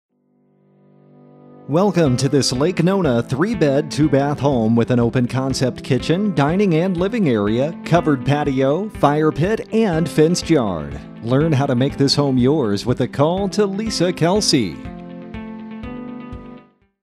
11981 Charades Street Orlando, FL | ColdwellBankerHomes.com
11981 Charades Street, Orlando, FL

Theres a lot to see inside this stunning home in Lake Nona. Built in 2020, featuring three bedrooms, two bathrooms and a bonus room/flex space. The open, bright and light floor plan features a spacious and stylish kitchen with quartz countertops and a large island open to the beautiful dining and l